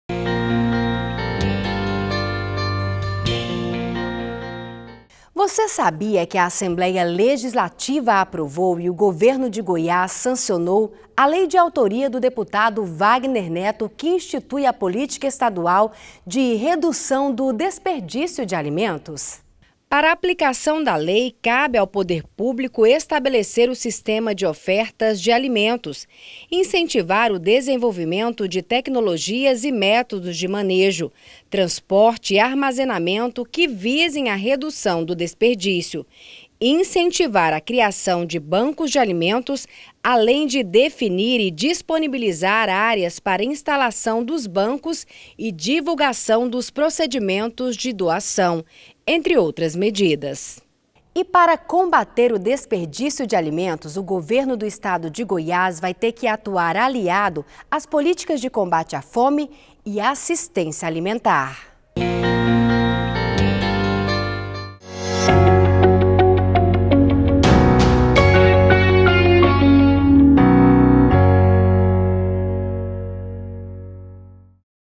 Você sabia que É LEI ? Redução do desperdício de alimentos em Goiás - Dep.Wagner Neto - 04/08/2022
Foi sancionada pelo governador Ronaldo Caiado (UB) a Lei Estadual nº 21.518 (originalmente projeto de lei nº 4732/19), de autoria do deputado Wagner Neto (PRTB), que institui a política estadual de redução do desperdício de alimentos em Goiás.

Reportagem de Edriza Borges e equipe TV Alego.